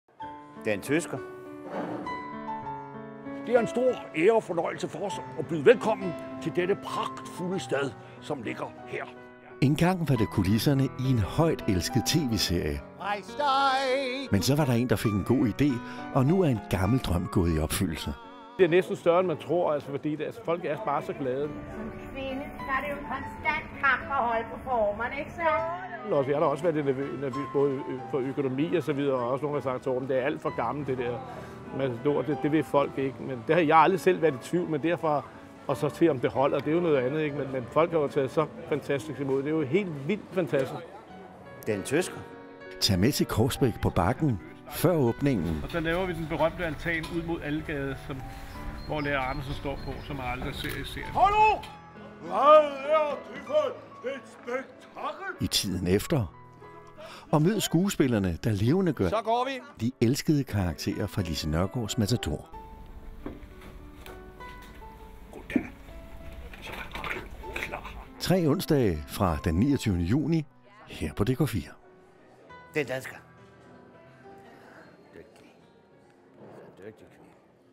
Korsbæk på Bakken
I foråret 2015 åbnede en ny attraktion på Bakken; ”Korsbæk på Bakken”.
En lille by, der er taget ud af Lise Nørgaards klassiker Matador.
100 millioner er der investeret i projektet for at skabe et univers, som ligger så tæt på tv-serien som muligt.

Kom med bag kulissen i "Korsbæk på Bakken"